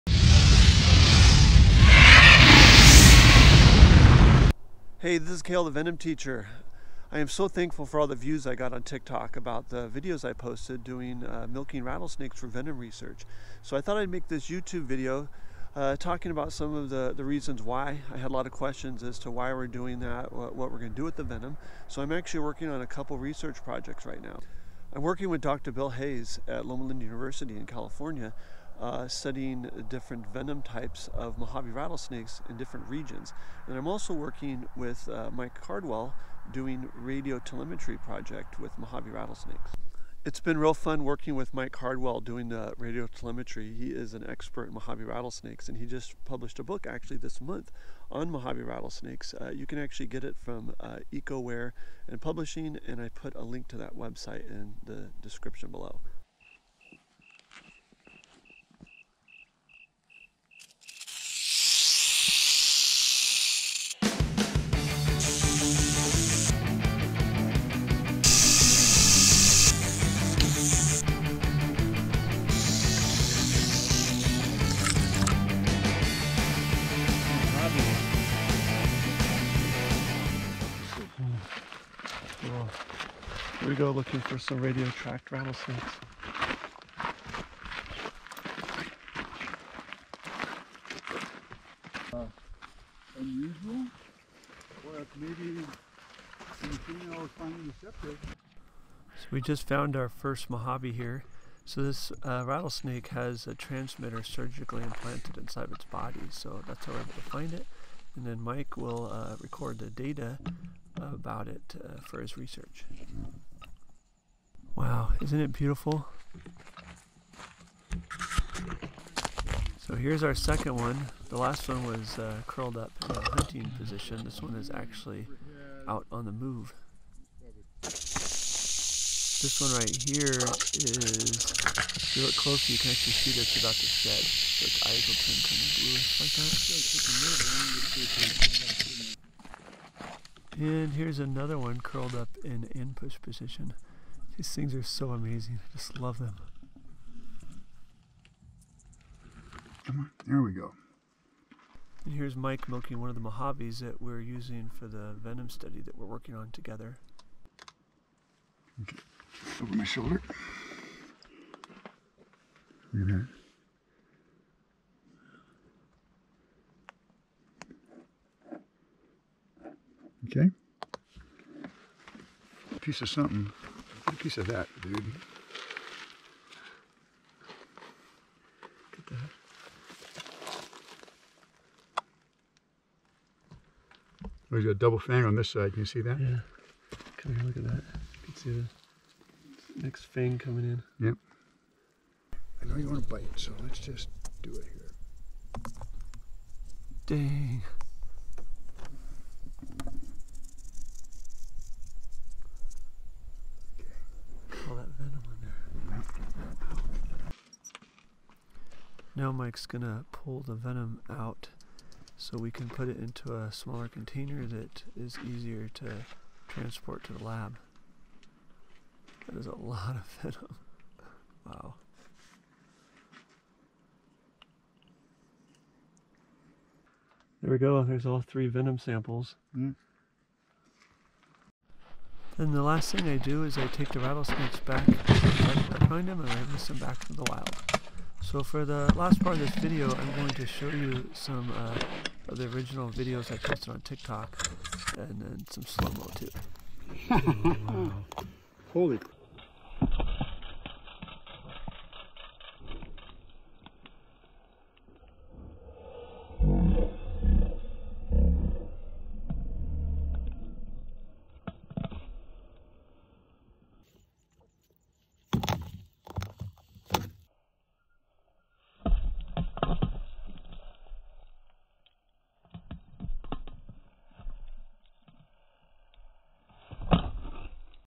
Mohave Rattlesnake Research
See what its like doing research with Mohave Rattlesnakes.
Get Mike Cardwell's new book "The Mohave Rattlesnake" online at Eco Wear and Publishing. Here's the link.